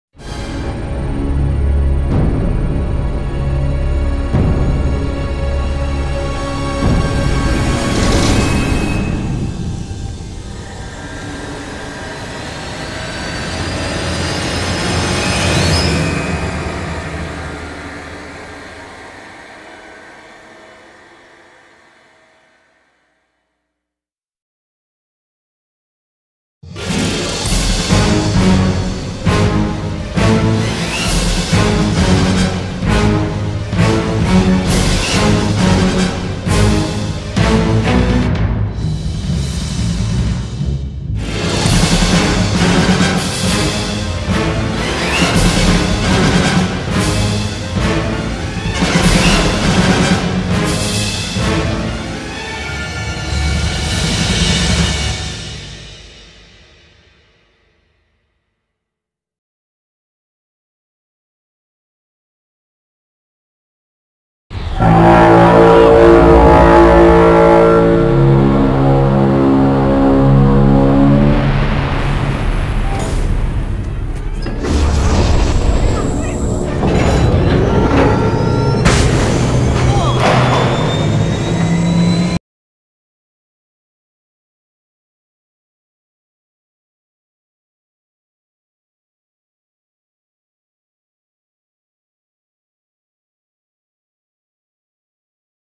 war of the worlds tripod visual effects test
just a -not so good- war of the worlds tripod visual effects test

tell me ur opinions

oh and by the ways don't think that i forgot to do disaster videos .i just have some stupid exams after finishing .u should get ready for a METEOR SHOWER OF DISASTER VIDEOS !